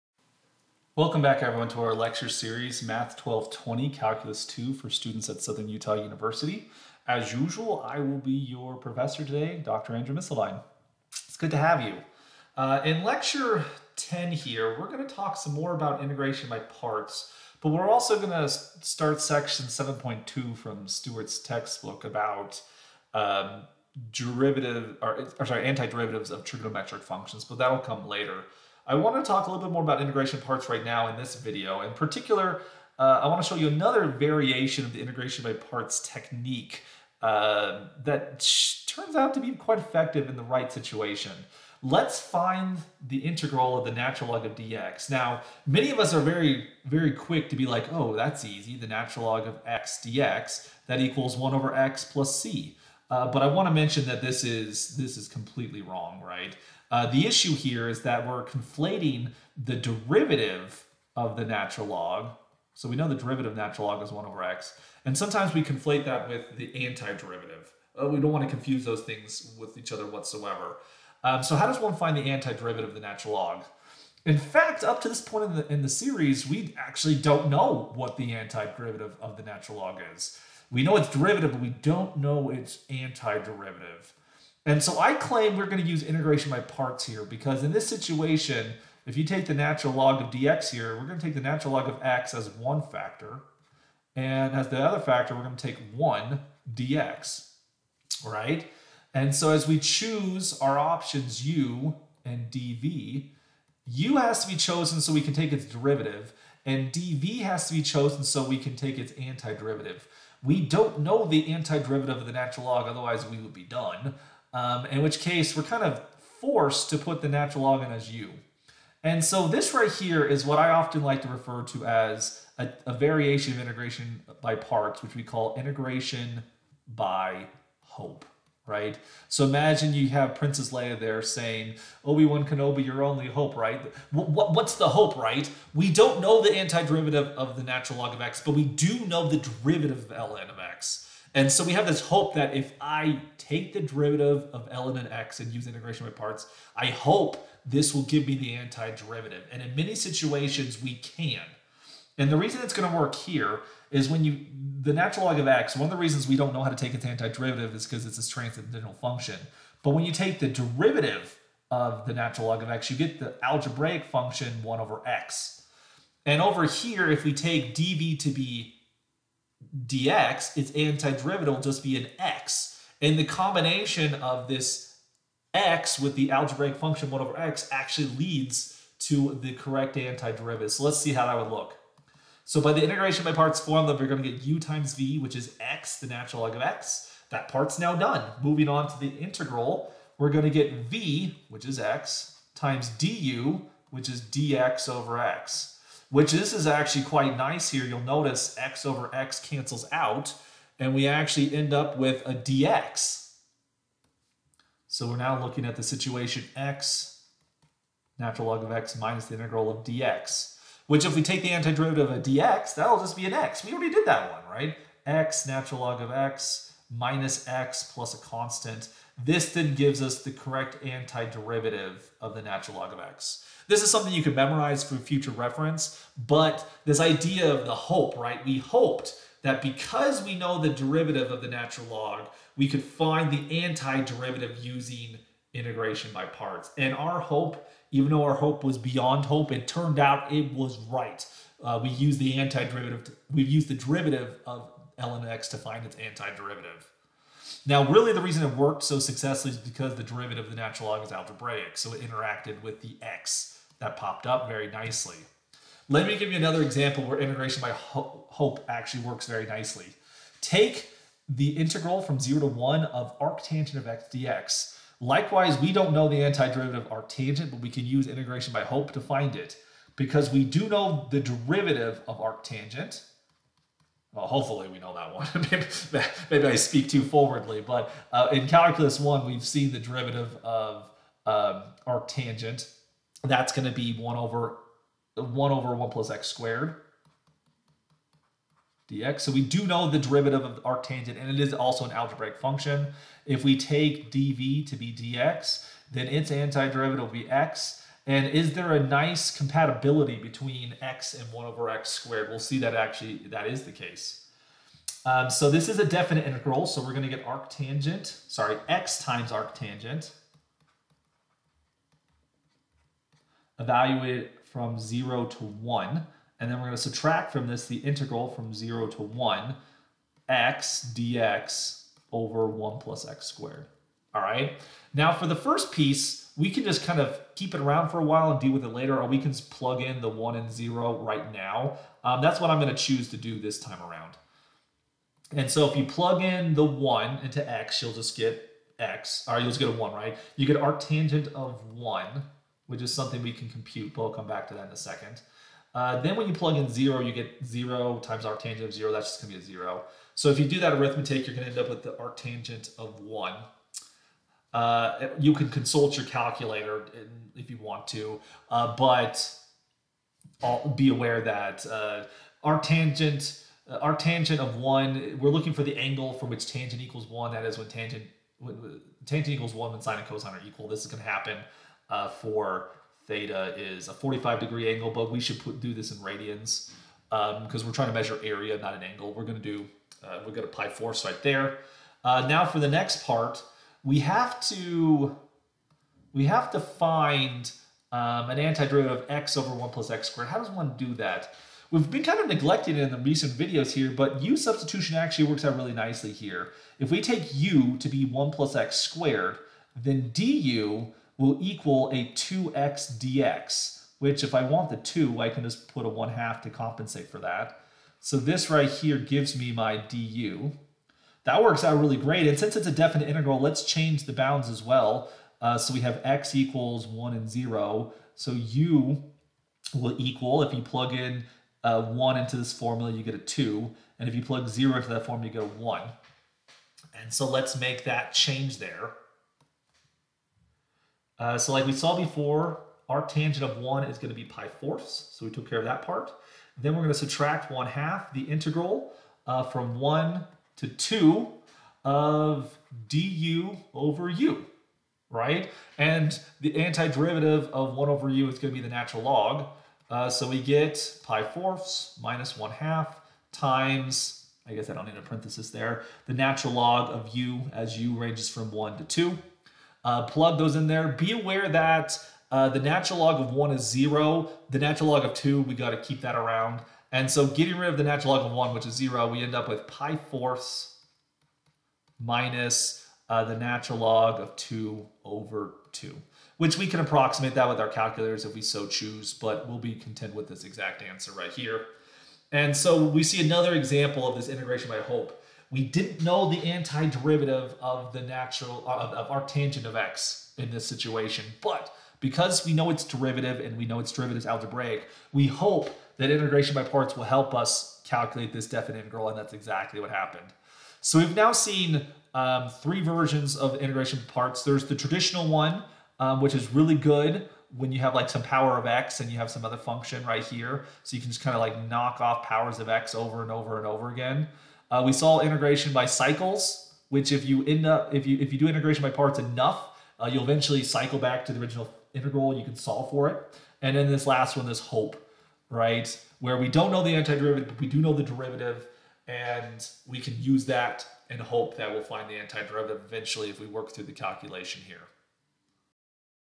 Integration by Hope
In this video, we introduce the technique of Integration by Hope.

This is lecture 10 (part 1/5) of the lecture series offered by Dr. Andrew Misseldine for the course Math 1220 - Calculus II at Southern Utah University. A transcript of this lecture can be found at Dr. Misseldine's website or through his Google Drive